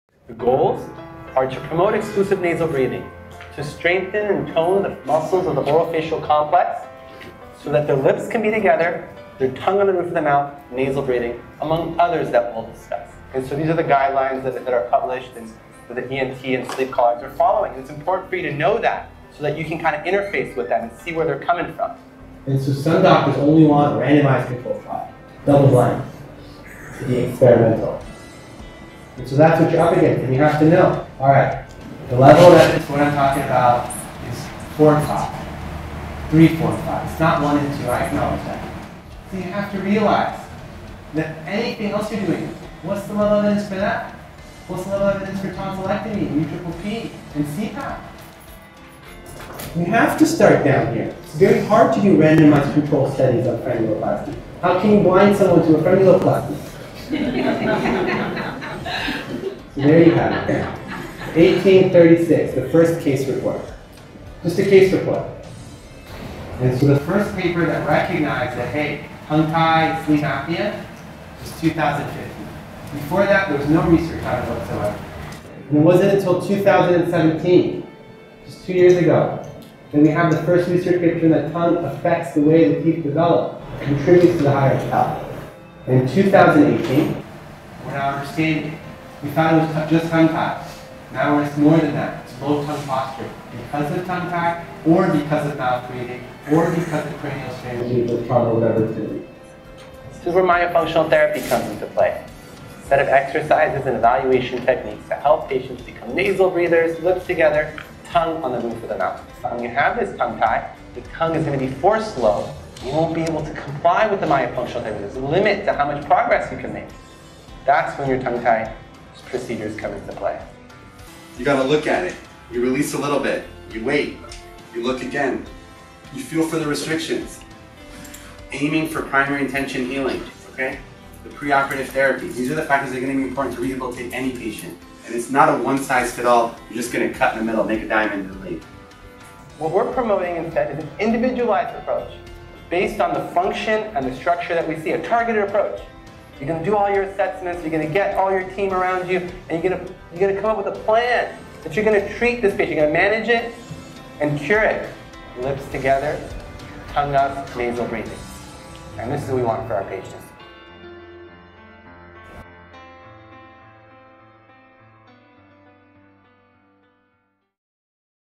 Functional Approach to Sleep and Breathing + Frenuloplasty Course (AUSTRALIA APRIL 2019)
Only a few days left to register for our 2019 Functional Approach to Sleep and Breathing + Frenuloplasty Course (AUSTRALIA APRIL 2019).

On April 27th-28th, 2019 Dr. Soroush Zaghi, Cassandra Carlopio, and Sarah Beach of The Breathe Institute will be in Sydney Australia to host two content rich days of lecture, workshop and demonstration focused on the precision diagnosis and treatment of sleep disordered breathing and airway issues through a multidisciplinary lens. to learn more and register today!